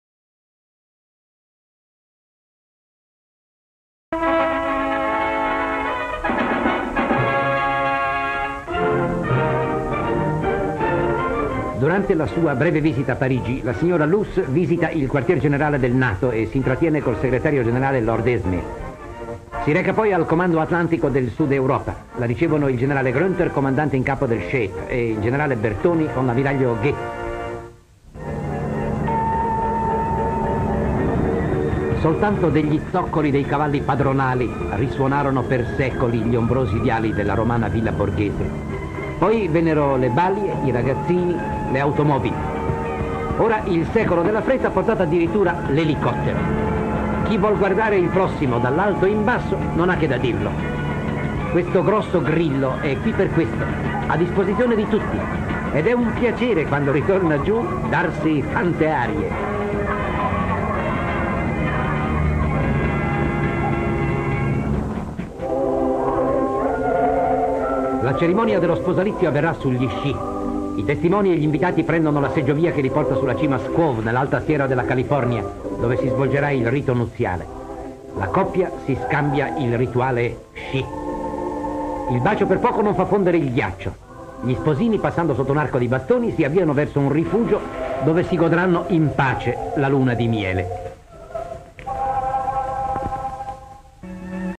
Colpi d'obiettivo. Parigi. Roma. California.
Descrizione sequenze:Quartier Generale della Nato a Parigi ; la signora Luce con il segretario generale lord Ismey ; poi si reca al comando Atlantico del Sud-Europa, i fotografi la riprendono insieme al generale Gruenther, poi al comandante Bertoni e l'ammiraglio Ghe ; Roma, villa Borghese: un elicottero decolla con alcuni bambini a bordo, una folla ne segue i movimenti ; l'elicottero sorvola villa Borghese poi atterra ; California: invitati ad un matrimonio raggiungono il luogo della celebrazione con la seggiovia ; il rito viene celebrato sugli sci: la sposa è in abito bianco, intorno gli invitati con giacche a vento e passamontagna ; bacio tra gli sposi, poi gli sposi passano sotto alle racchette sorrette ada arco ; gli sposi scendono sugli sci per raggiungere un rifugio ;